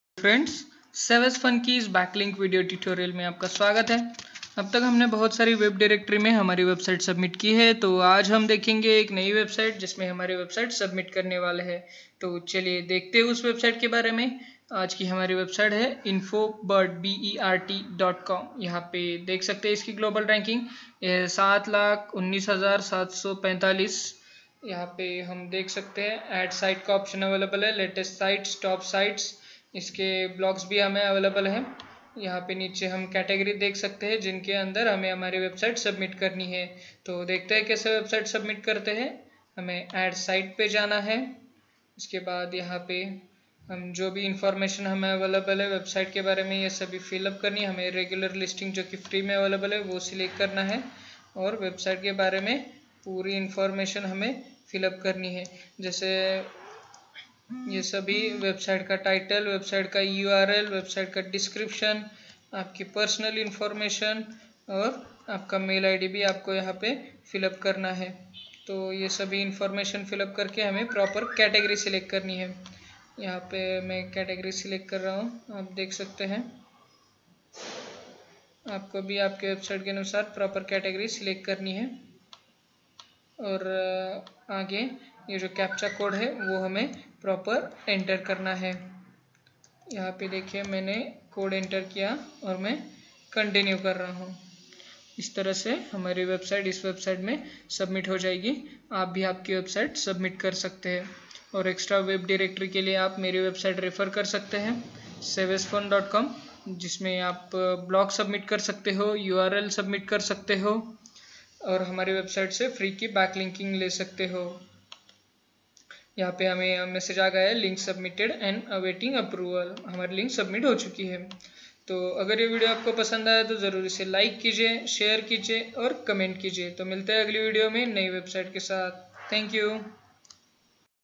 HOW TO BACKLINK ON infobert.com BY SAVESFUN.COM
WELCOME to this new backlinking video. we are getting backlink in the website shown below in description and step by step procedure is also mention there.

Procedure:-
1. Goto home page on infobert.com
2. select option Add site.
3. choose price list(select free).
4. fill all information mention there.
5. select category from list.
6. enter correct captcha.
7. submit.

you wil get successfully listing message on screen.